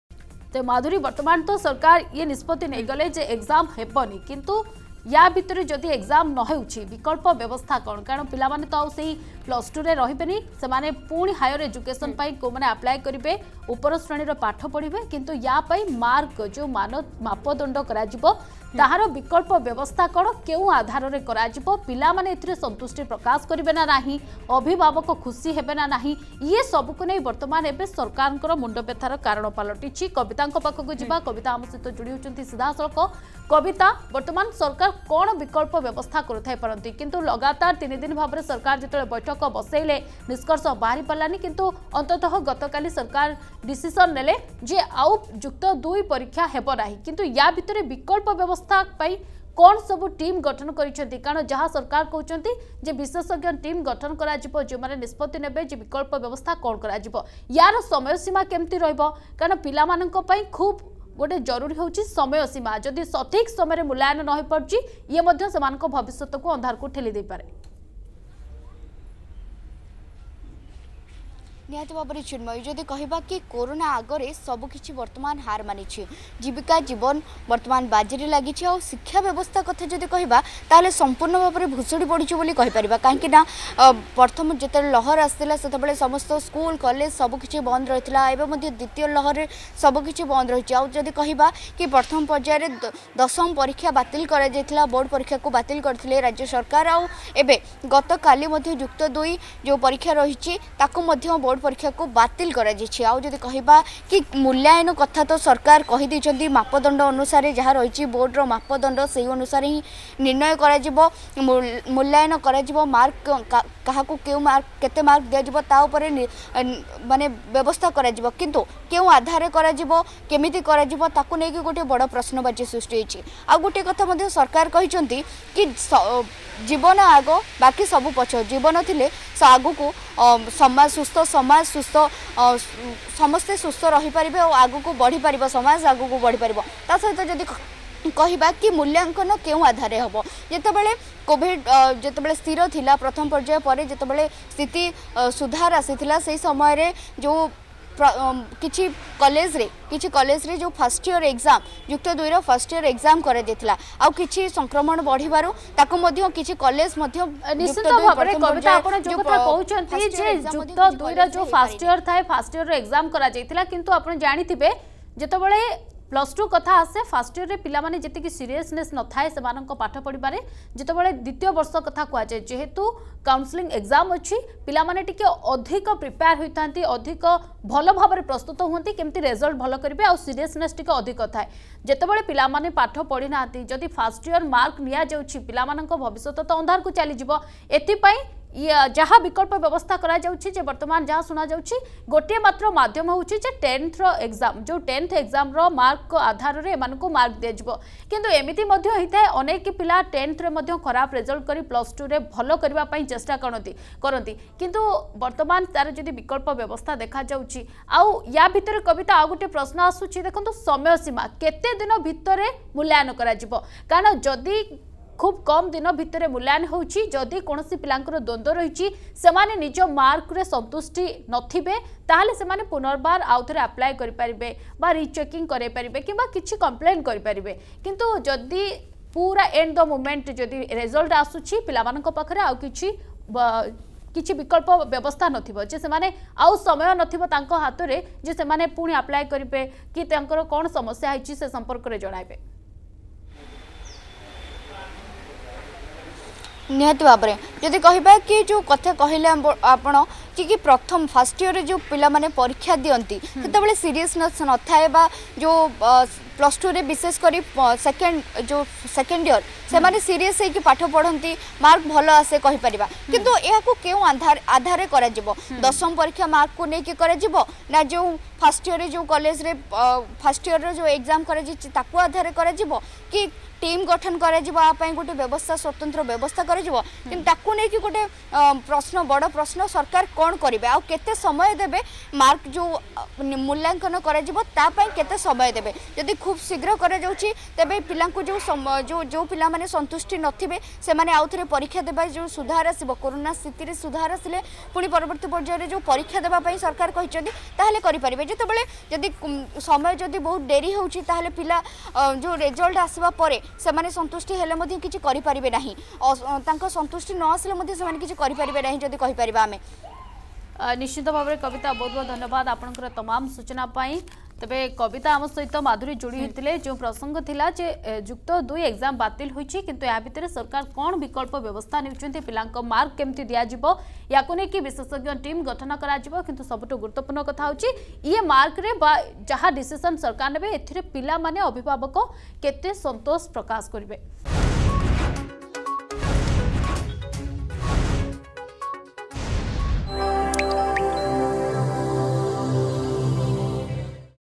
ଯୁକ୍ତ ୨ ପରିକ୍ଷା ବାତିଲ ଘୋଷଣା କଲେ ମୁଖ୍ୟମନ୍ତ୍ରୀ
ଯୁକ୍ତ ୨ ପରିକ୍ଷା ବାତିଲ । ବିଶେଷଜ୍ଞଙ୍କ ପରାମର୍ଶ ଆଧାରରେ ହେବ ମୁଲ୍ୟାୟନ। ନିର୍ଦ୍ଦିଷ୍ଟ ସମୟରେ ମୁଲ୍ୟାୟନ କରିବାକୁ ନିର୍ଦ୍ଦେଶ ।